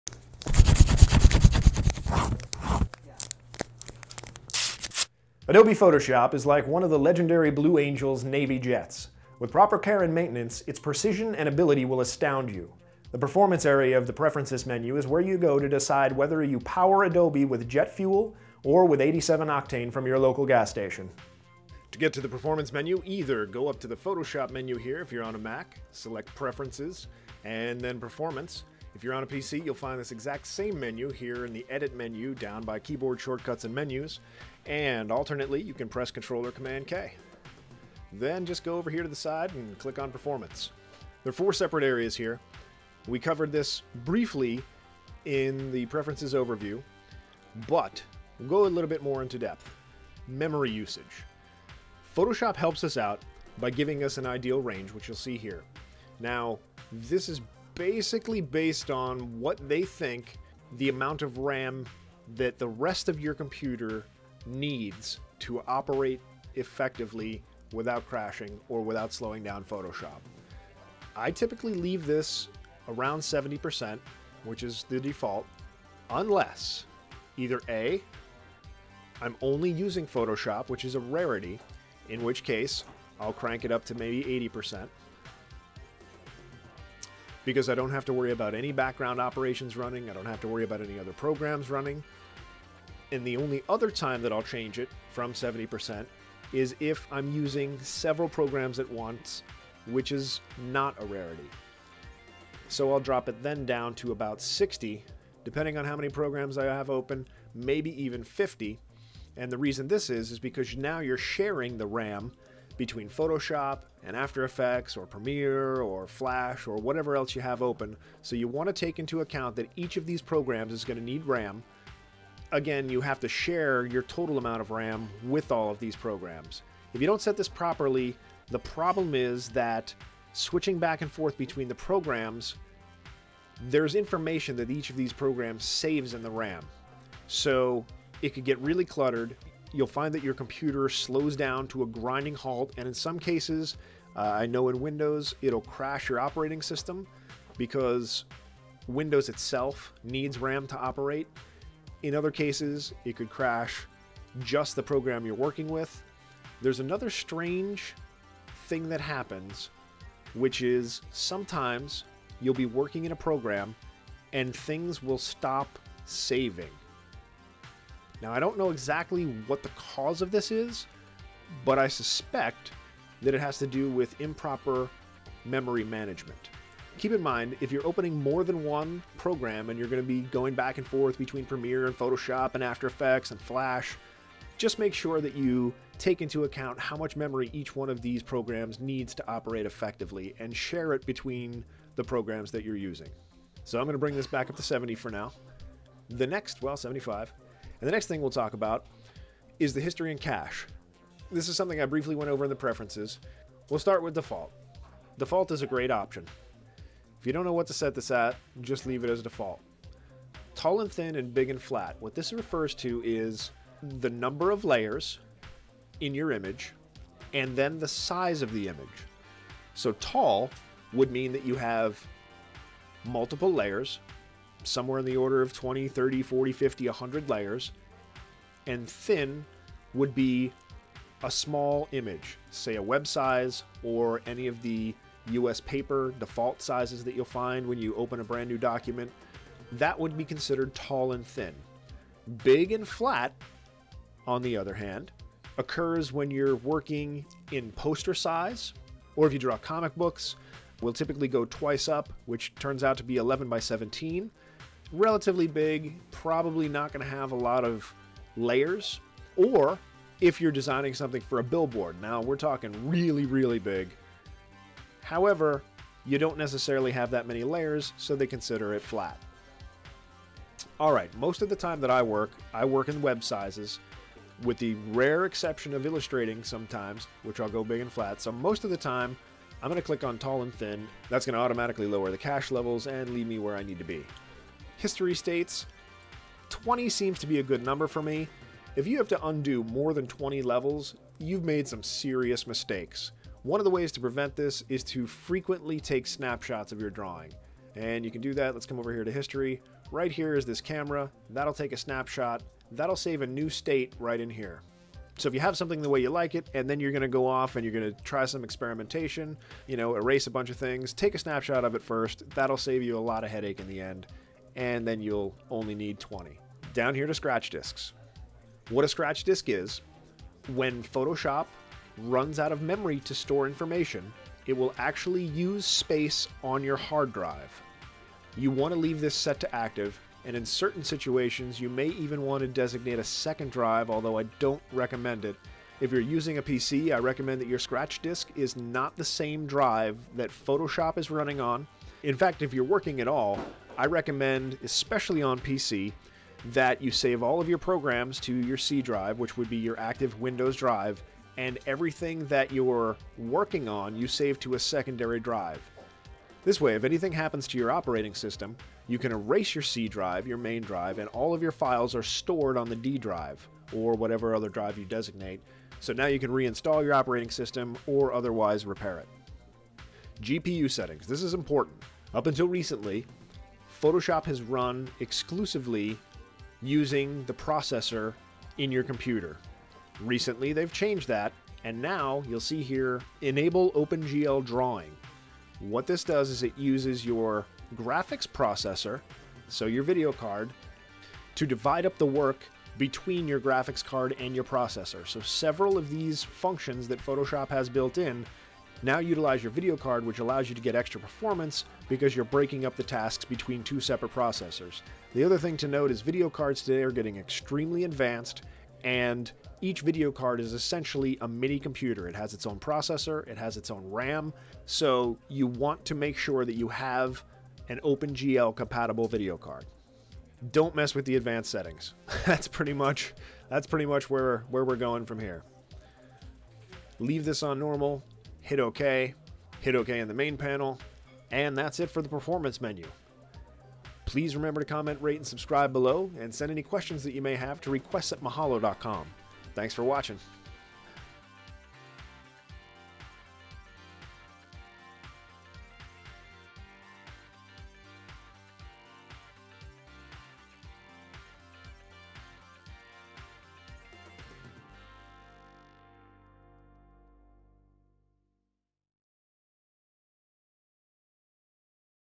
Learn Adobe Photoshop - Performance Preferences
In this video, Mahalo expert Gene Shaw explains the various settings and options in the Performance tab under Preferences.

Photoshop has several options that can be changed to optimize its performance. These can be changed in the Performance tab of the Preferences panel.
1. Open the Preferences menu by pressing Ctrl+K (Command+K for Mac) and click on the tab Performance.2. One thing you'll want to focus on here is Memory Usage. Photoshop helps you out by giving you an ideal range. This number is based on what Photoshop thinks is the amount of RAM that the rest of your computer needs to operate effectively without crashing or without slowing down Photoshop. 70% is the default. It might be a good idea to change this if Photoshop is the only program you're using or going to use (which is most likely a rarity), in which case 80% is a good increase. Otherwise, if you're using several programs at once, a decrease to 60% or even 50% is a good idea. It all depends on your personal computing habits. Basically, if you're switching back and forth between multiple programs, it's a good idea to keep in mind how much memory each program is using so your computer can run as efficiently as possible. 3. If you don't know a good setting for the History & Cache, then Default is probably your best option. The other two options are Tall & Thin or Big & Flat. These refer to the number of layers in your image, and the size of your image.
* Tall & Thin is a good setting if you're working with multiple layers (20+) and a small image (web size or U.S. paper size).
* Big & Flat is a good setting when you're working with a poster-sized image (11x17), a billboard-sized image, or an image with few layers.

4. 20 is a good number for History States. This setting allows you to go a number of steps backward in case mistakes are made.5. When Photoshop runs out of memory to store information, it will begin using space on your hard drive. So, you want to make sure the box under Scratch Disks is checked Active.6. Up until recently, Photoshop has run exclusively using the processor in your computer. Now they have changed that, and you'll see Enable OpenGL Drawing. This new method uses the graphics processor (video card) in your computer to divide up your work between your graphics card and your processor. You'll want to have this box checked. Also, if you click Advanced Settings below Enable OpenGL Drawing, make sure that is set to Normal.